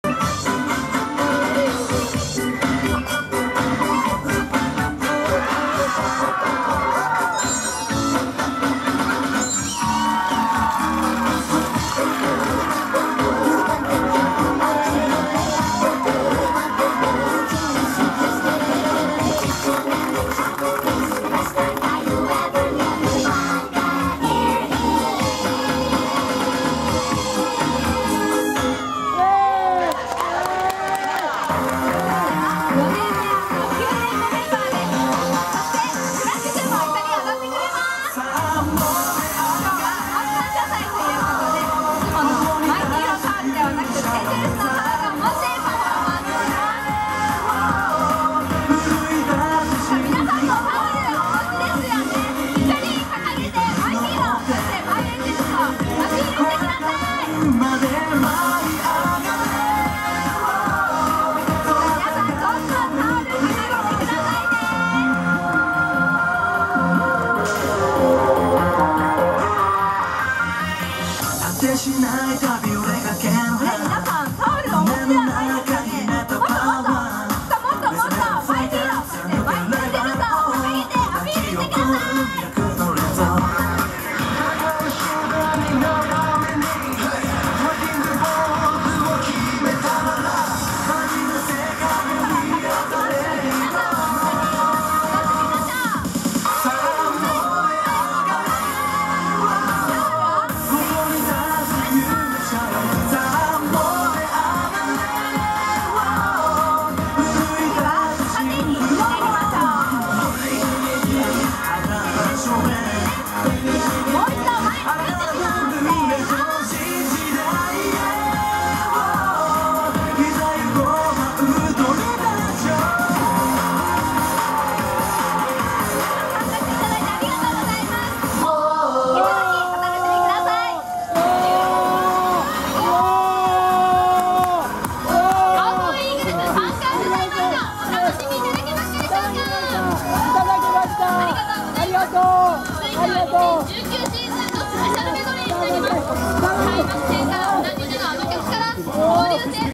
２０１９年11月24日楽天イーグルスファン感時のクラッチくん
この踊りは生で初めて見ました。クラッチくん、行動からダンスから全てがかわいい(*^-^*)